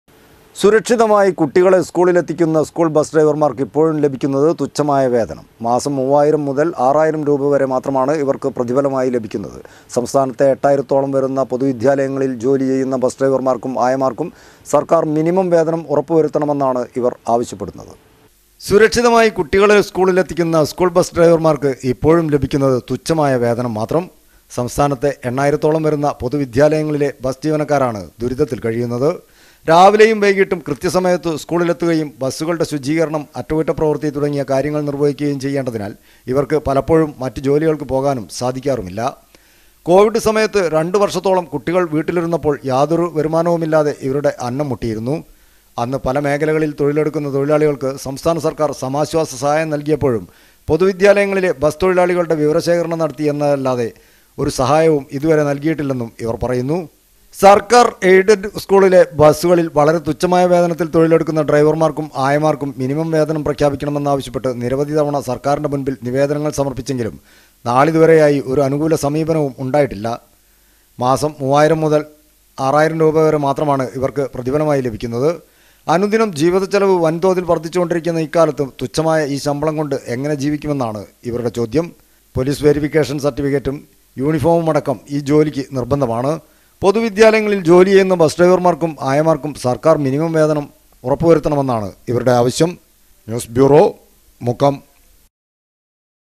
സ്കൂൾ ബസ് ഡ്രൈവർമാർക്ക് ഇപ്പോഴും ലഭിക്കുന്നത് തുച്ഛമായ വേതനം. മാസം 3,000 മുതൽ 6,000 രൂപ വരെ
സുരക്ഷിതമായി കുട്ടികളെ സ്കൂളിൽ എത്തിക്കുന്ന സ്കൂൾ ബസ് ഡ്രൈവർമാർക്ക് ഇപ്പോഴും ലഭിക്കുന്നത് തുച്ഛമായ വേതനം. മാസം 3,000 മുതൽ 6,000 രൂപ വരെ മാത്രമാണ് ഇവർക്ക് പ്രതിഫലമായി ലഭിക്കുന്നത്.